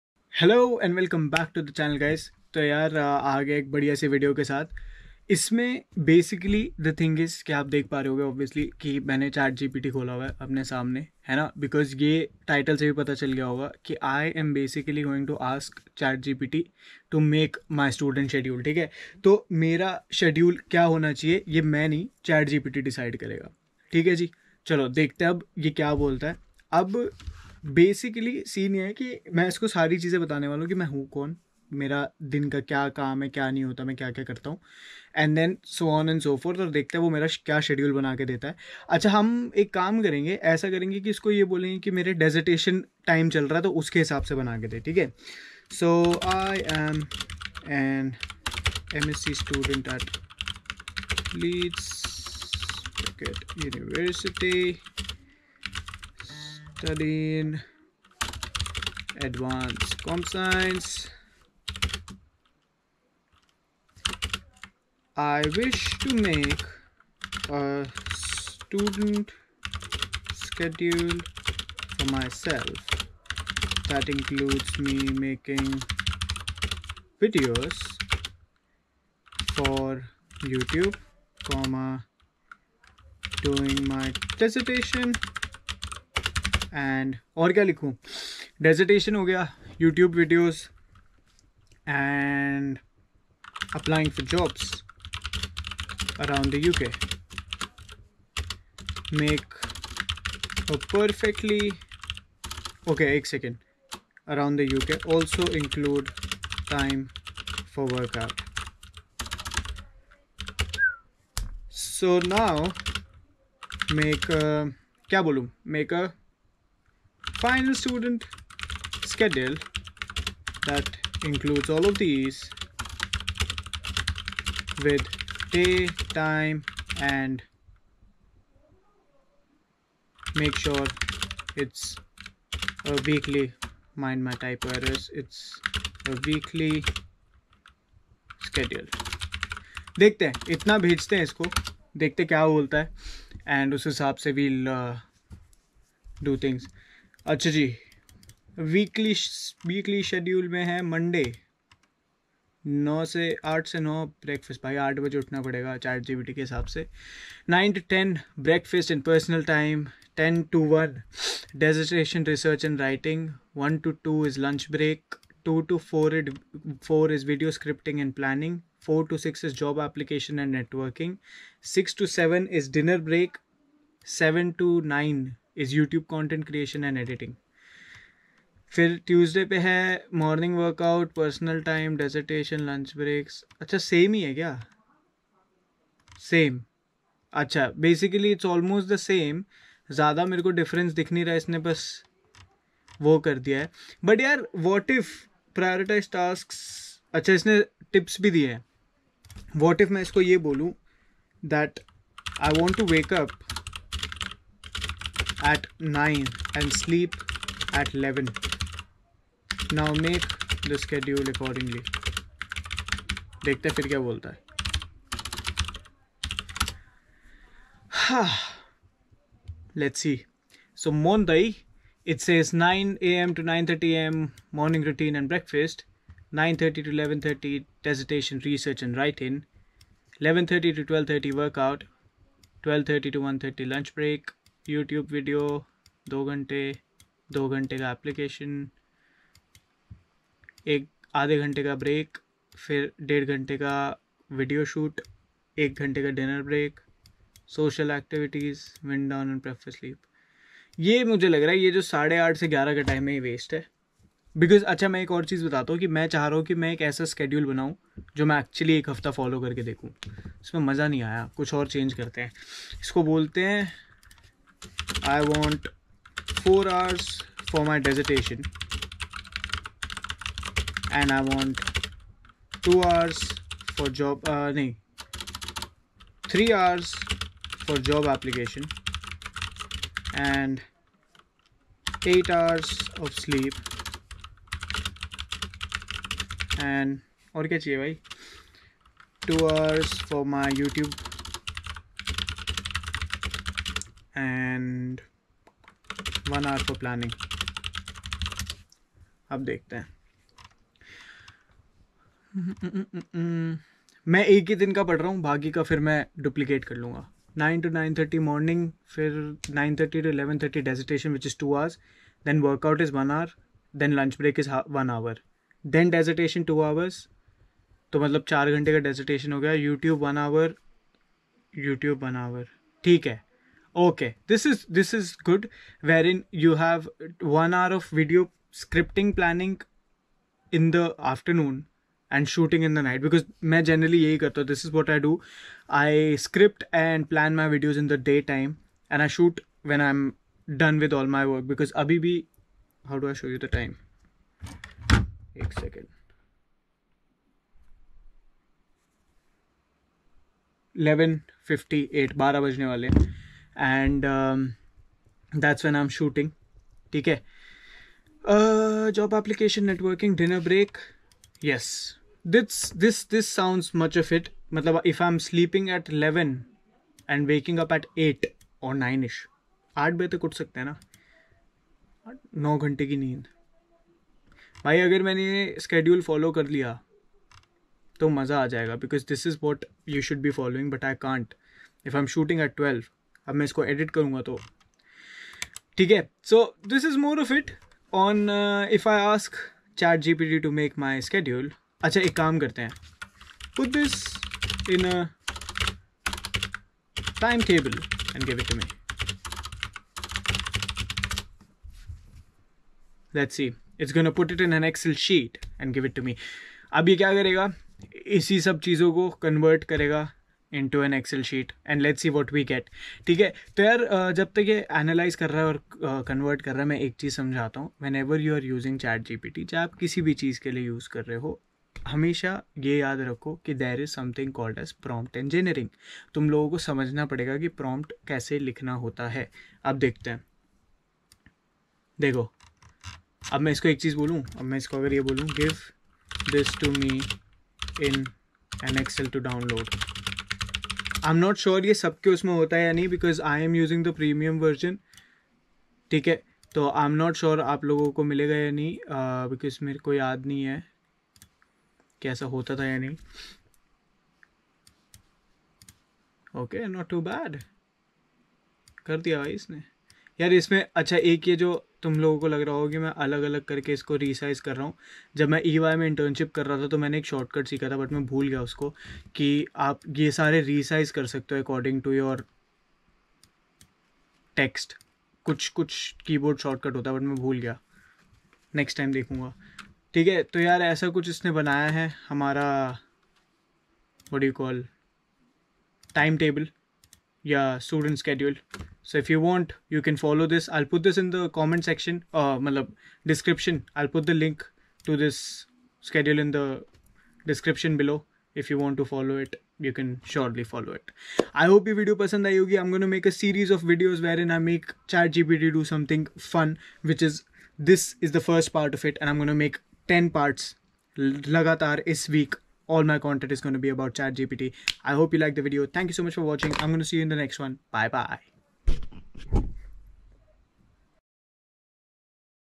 I Asked ChatGPT to Plan My Study Schedule: Here's What Happened!
Description:
Ever wondered how to create the perfect study schedule? I asked ChatGPT to help me design a study plan tailored for university students. Watch as I follow the AI's advice and see how it transformed my study routine. Spoiler: It involves way more breaks than you'd think!

Study schedule, ChatGPT, time management, university students, productivity hacks, AI planning.